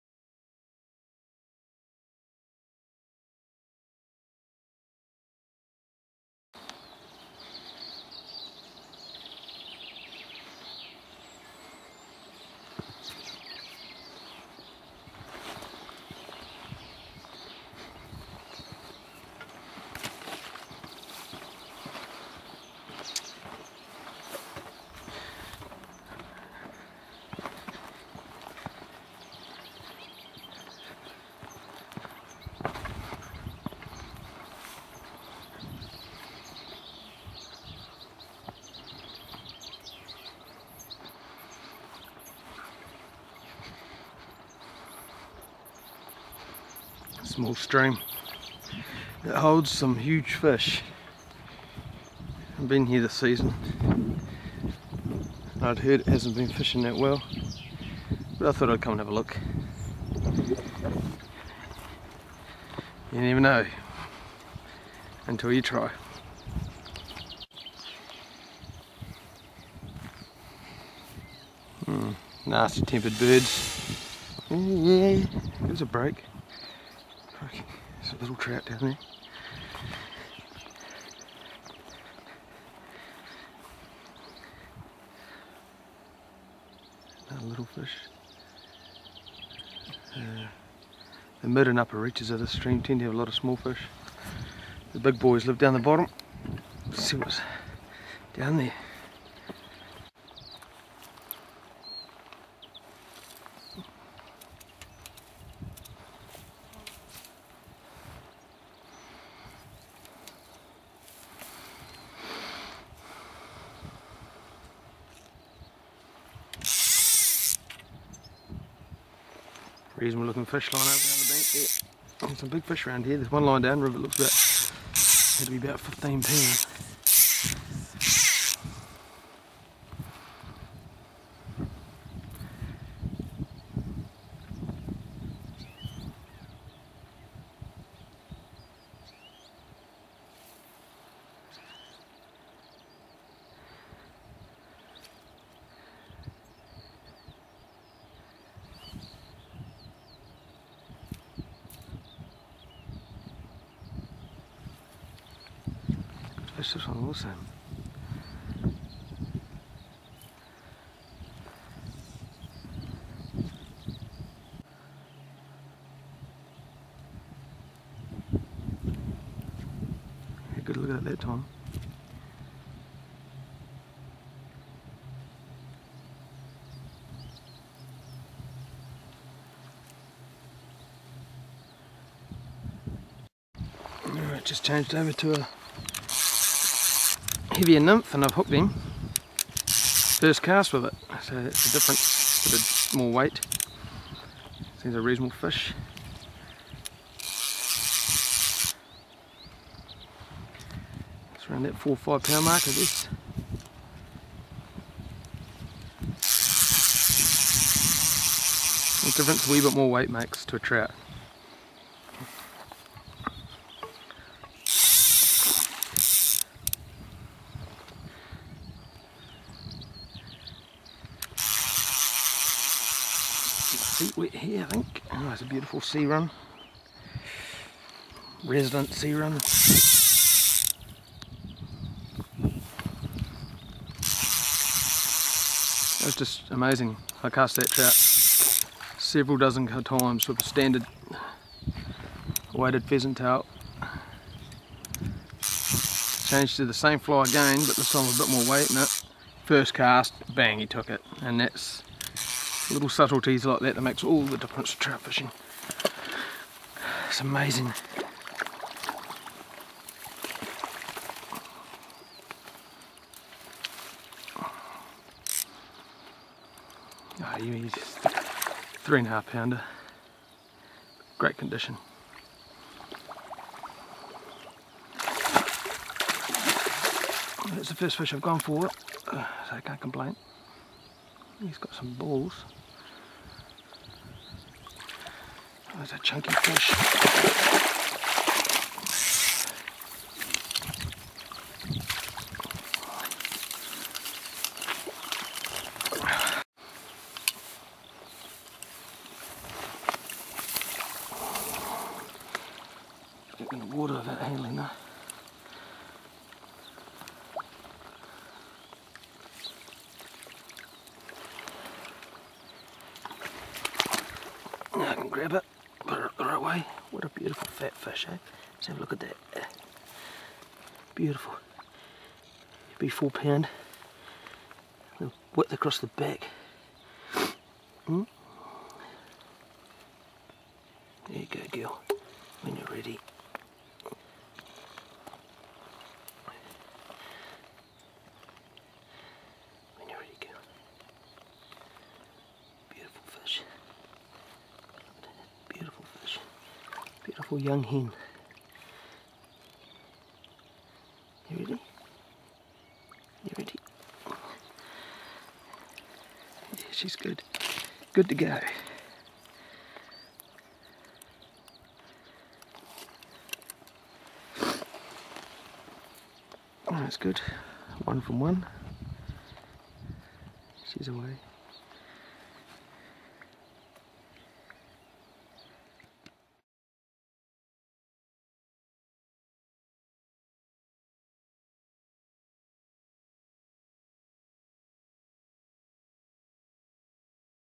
Fly fishing in New Zealand...Spring creek sea run
Sight fishing to a nice sea run trout on a local stream...All tackle supplied by the Fishermans' loft